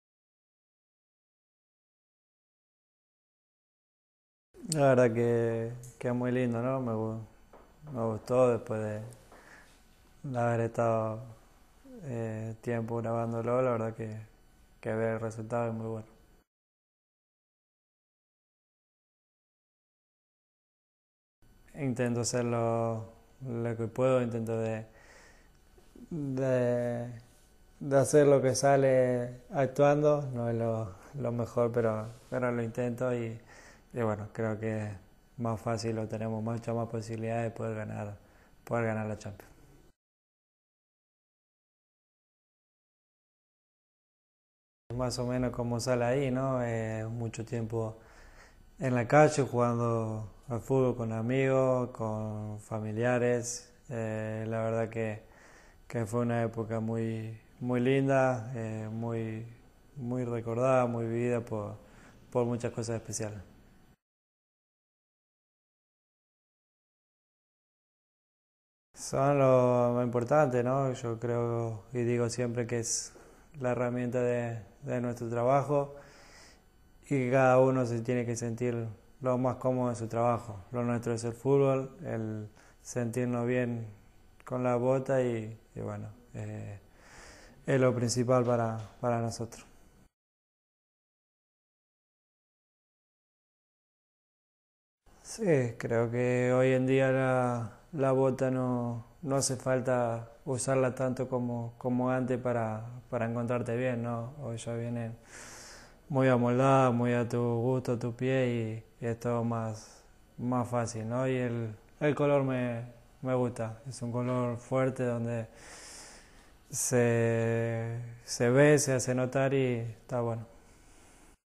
Barcelona star Lionel Messi
MPEG title: Lionel Messi IV 3 (SPANISH)

E: Do you like the new F50i commercial?
M: Actually, it's great, I think. After having been there for quite some time shooting it I now see the result of it and it's great.
M: La verdad es que es muy lindo, no? Me gustódespues de haber estado un tiempo mirandolo, y ahora que veo el resultado, es muy bueno.

Lionel Messi PR IV (Spanish)
E: What is easier: to win an Oscar or to win the Champions League?
M: I do what I can, I do my best as an actor. It's not amazing but I really try and .. well, I think we have it far easiermuch more chances to win the Champions League.
M: Intento hacer lo que puedo, intento hacer lo que sale actuando. No es lo mejor, pero lo intento y bueno..creo que más fácil lo tenemos  mucho más posibilidades para poder ganar la champions.

E: In the commercial we see a young Lionel Messi playing the the streets with his friends. How was your childhood in Rosario?
M: Well, more or less like you can see in the spot, you know. A lot of time in the streets with my friends and acquaintances. It was actually a beautiful time which I love to remember, which was very lively for a lot of reasons.
M: Sí, más o menos como sale allí no? Mucho tiempo en la calle jugando con los amigos, con familiares. La verdad es que fue una epoca muy linda, muy recordada, muy viva por muchas cosas especiales.

E: How important are your boots?
M: They are the most important thing for a footballer. I always say it is the tool of our work and everybody has to feel as comfortable as possible in his work. Our thing is to play football, to feel good in the shoesyes, that's the most important thing for us.
M: Son lo más importante no? Yo creo y digo siempre que es la herramienta de nuestro trabajo y cada uno se tiene que sentir lo más cómodo en ese trabajo. Lo nuestro es el fútbol, sentirnos bien con la bota y buenoes lo principal para nosotros.

E: Do you feel comfortable in these shoes? What do you like about them?
M: I believe that nowadays it is no longer necessary to wear a boot for a certain time in order to finally feel comfortable in it. The boots are very well shaped, very personalized, adjusted to your feet and that makes it easier. I really like the color of the F50i, it's a strong color that strikes the eye and calls the attention y I like that.
M: Sí, creo que hoy en día la bota no hace falta usarla tanto como antes para encontrarte bien, la bota viene muy amoldada, muy a tu gusto, a tu pie y esto es más fácil. Y el color me gusta, es un color muy fuerte, que se ve, que se hace notar y es muy bueno.